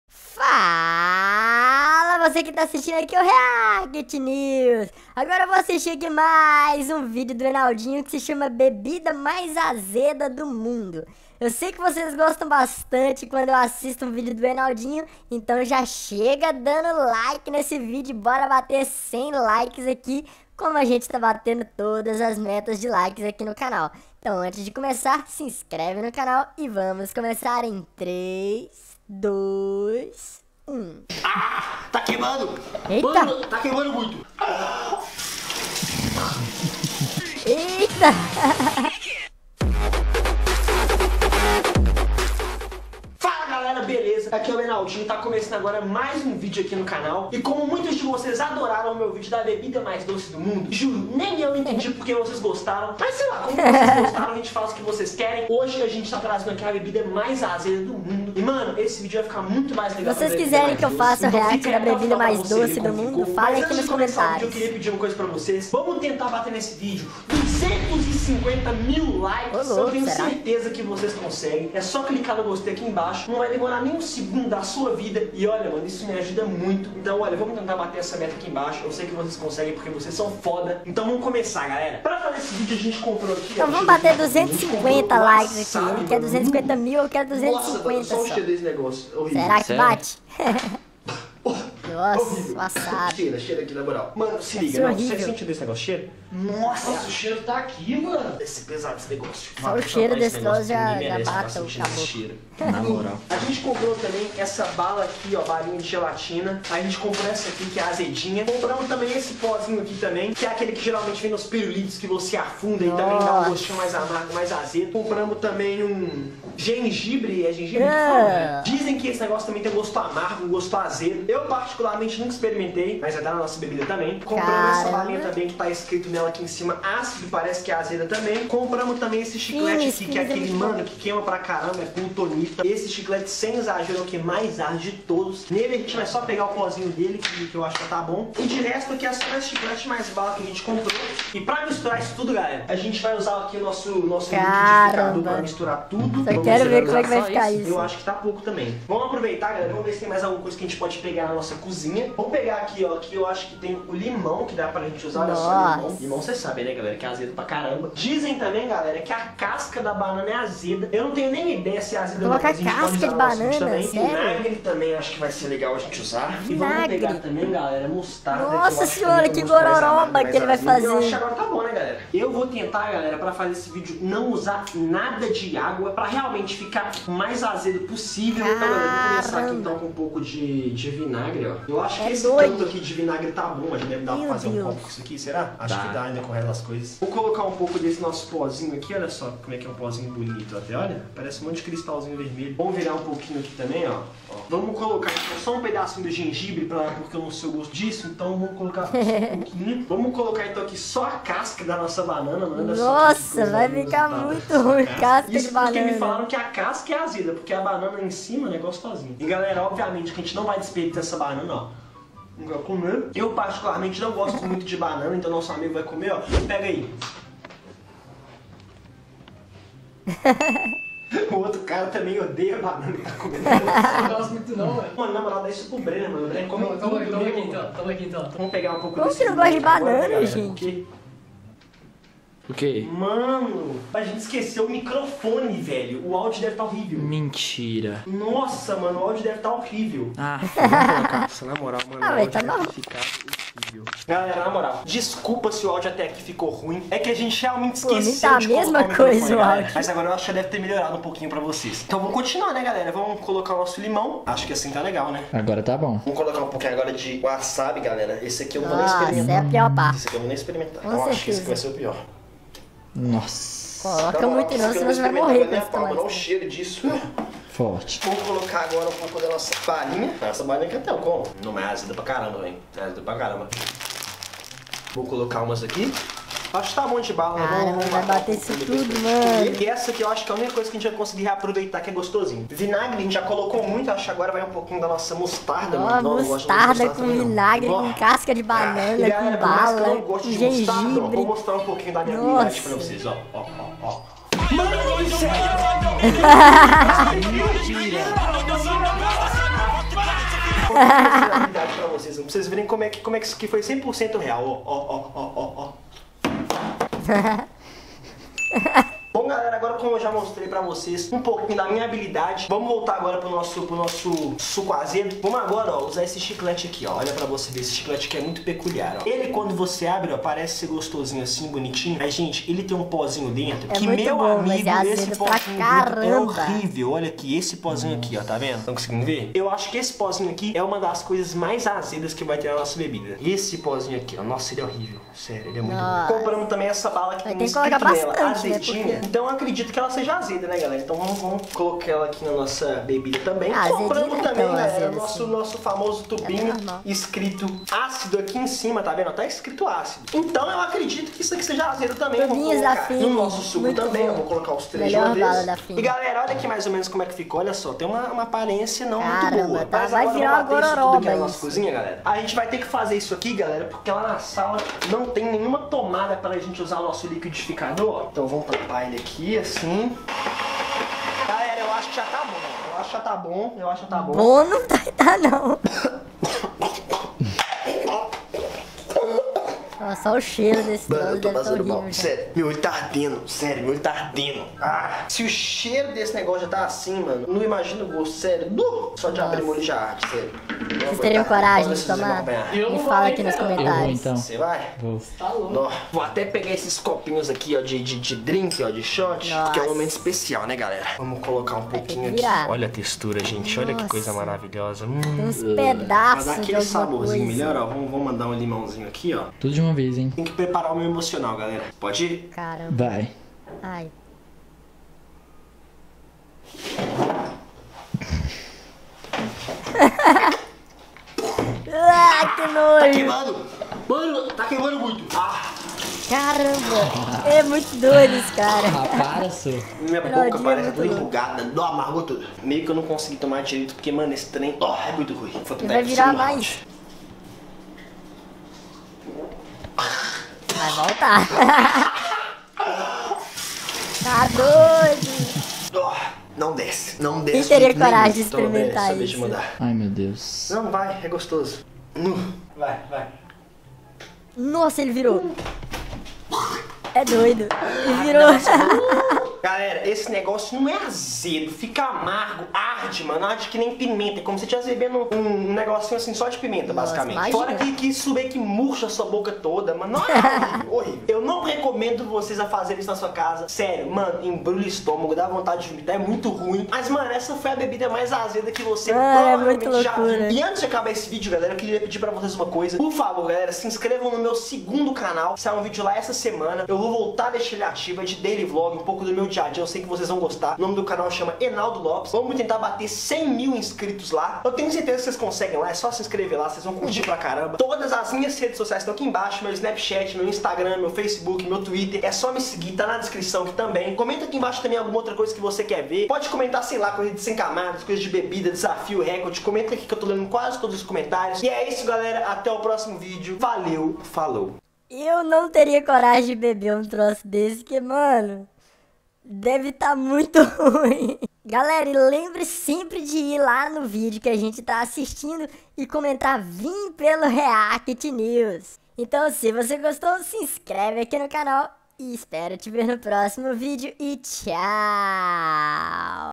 REACT - BEBIDA MAIS AZEDA DO MUNDO? (Enaldinho)
Vocês teriam coragem de beber isso que o Enaldinho criou? kkkk